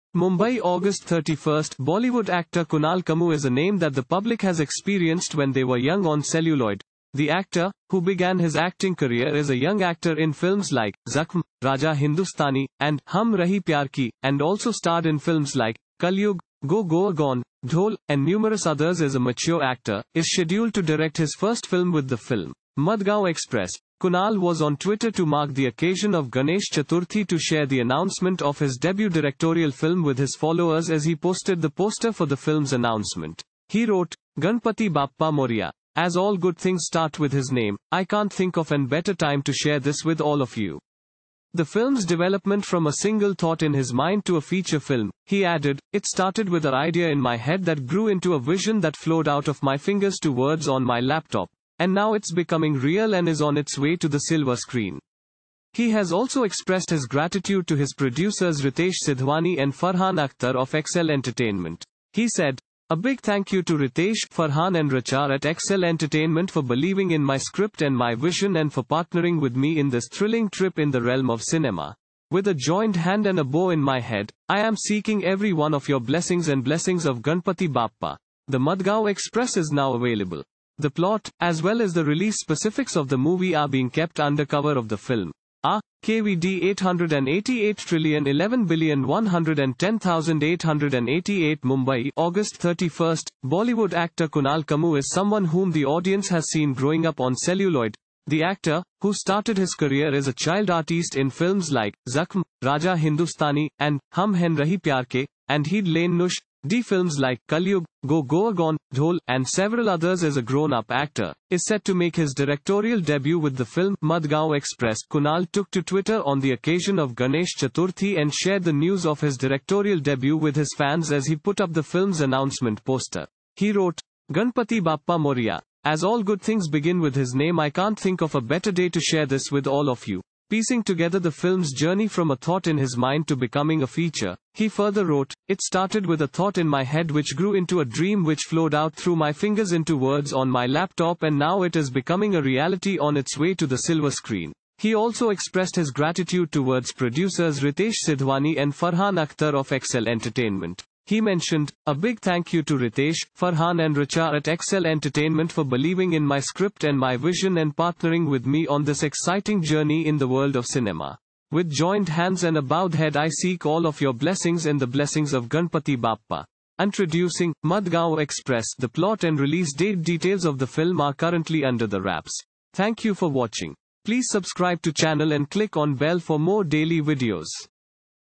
Kunal Kemmu To Make Directorial Debut With "Madgaon Express" | Movie
Mumbai Aug 31,  :  Bollywood actor Kunal Kemmu is a name that the public has experienced when they were young on celluloid.The actor, who began his acting career as a young actor in films like 'Zakhm', "Raja Hindustani', and 'Hum Rahi Pyar Ki', and also starred in films like "Kalyug," 'Go Goa Gone", 'Dhol' and numerous others as a mature actor, is scheduled to direct his first film with the film "Madgaon Express.".
Kunal was on Twitter to mark the occasion of Ganesh Chaturthi to share the announcement of his debut directorial film with his followers as he posted the poster for the film's announcement.He wrote: "Ganpati Bappa moriya.As all good things start with his name, I can't think of an better time to share this with all of you."

The film's development from a single thought in his mind to a feature film, he added: "It started with a idea in my head that grew into a vision that flowed out of my fingers to words on my laptop.And now it's becoming real and is on its way to the silver screen."

He has also expressed his gratitude to his producers Ritesh Sidhwani and Farhan Akhtar of Excel Entertainment.

He said: "A big thank you to Ritesh, Farhan and Rucha at Excel Entertainment for believing in my script and my vision and for partnering with me in this thrilling trip in the realm of cinema.With a joined hand and a bow in my head, I am seeking every one of your blessings and blessings of Ganpati Bappa.The Madgaon Express is now available.".

The plot, as well as the release specifics of the movie are being kept under cover of the film.